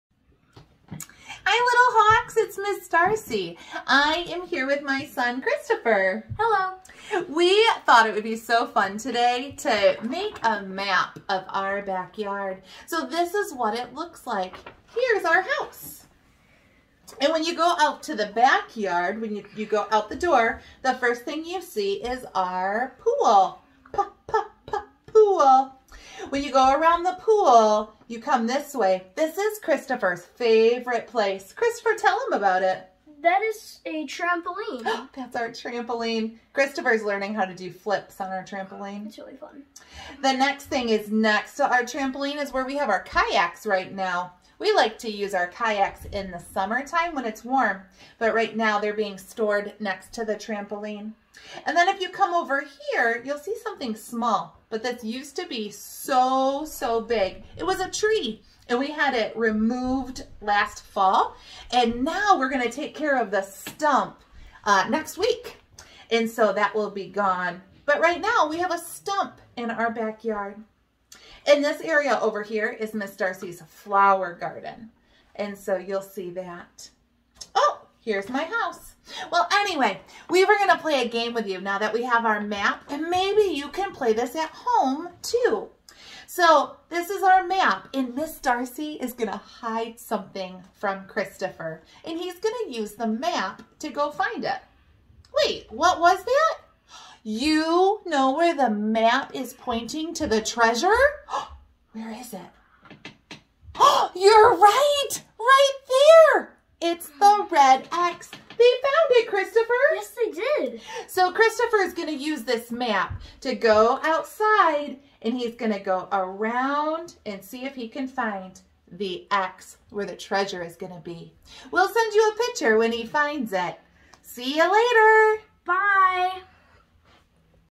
Map of your backyard treasure hunt
Make a map of your backyard.  Now take a stuffed animal to hide somewhere in your backyard.  Tell your family where the X is.  Follow the map to find the stuffed animal.  take a picture of you finding your treasure and send it to Ms. Darcy.